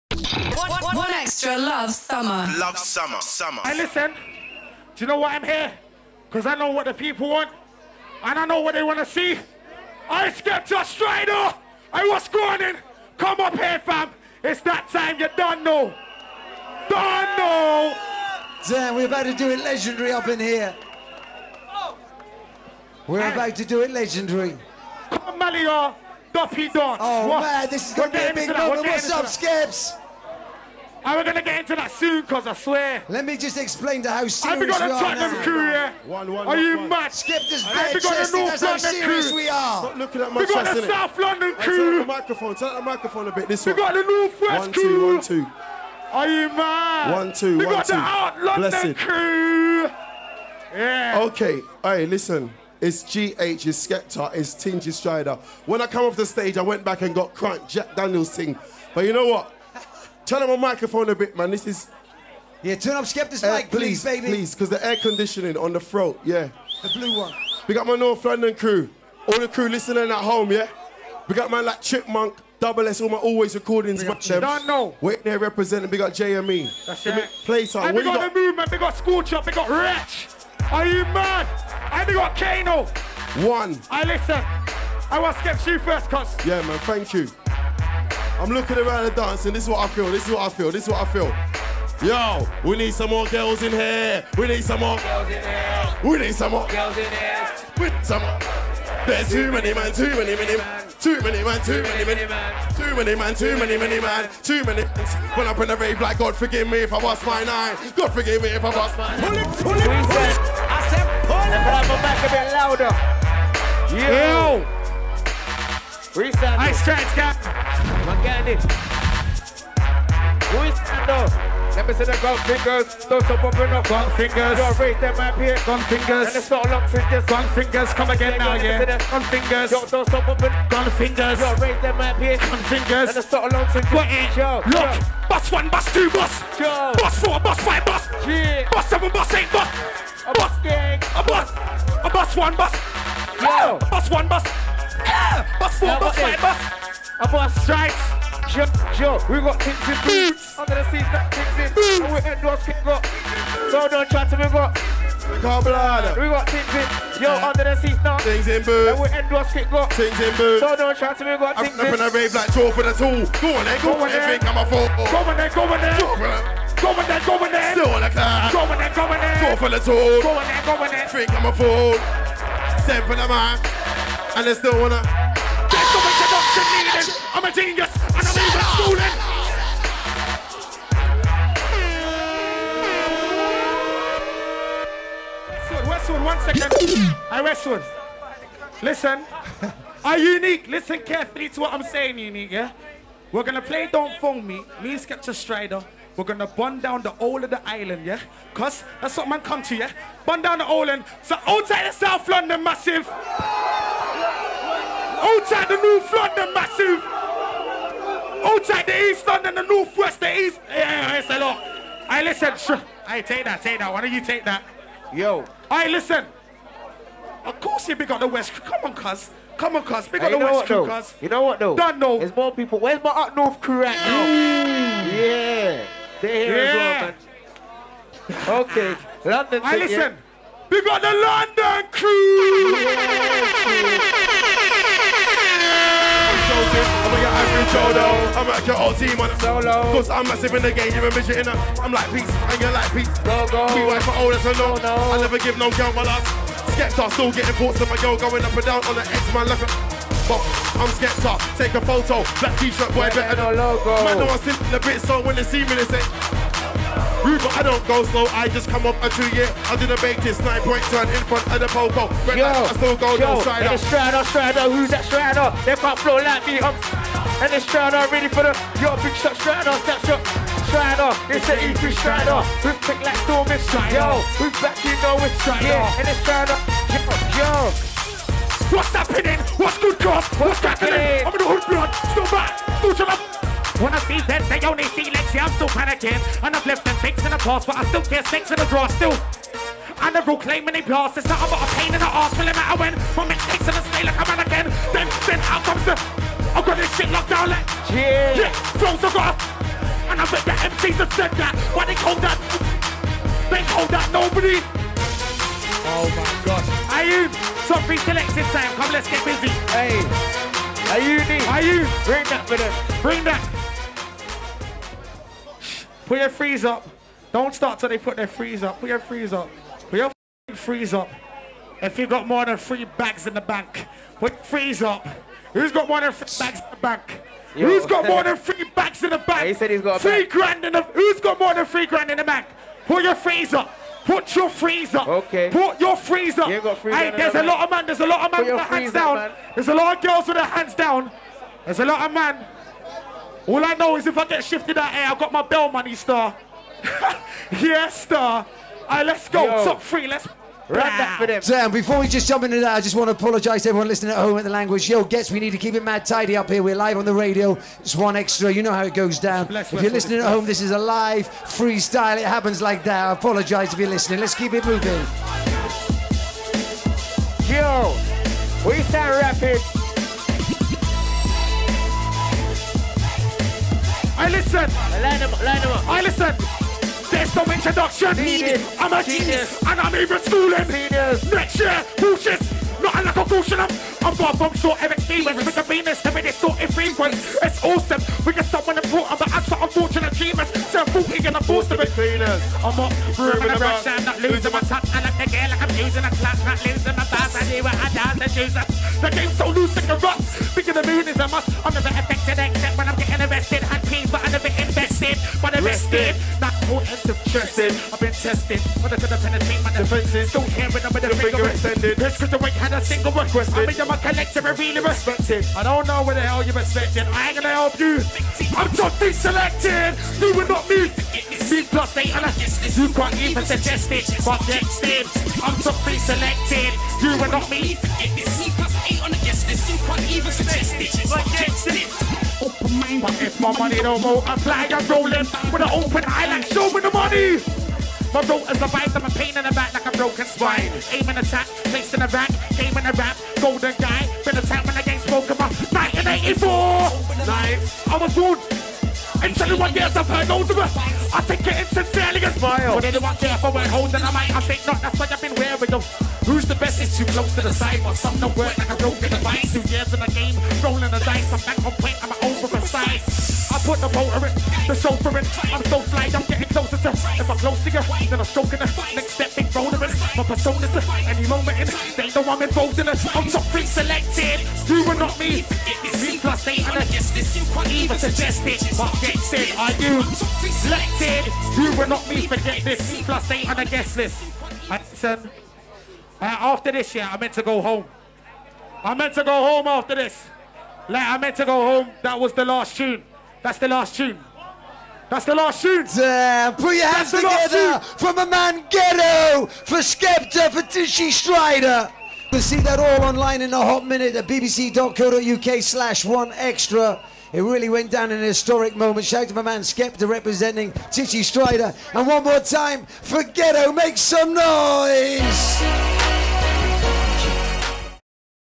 Tinchy, Skepta, Ghetto freestyle @ 1Xtra Beach Party
Tinchy, Skepta, Ghetto freestyle @ 1Xtra Beach Party in Malia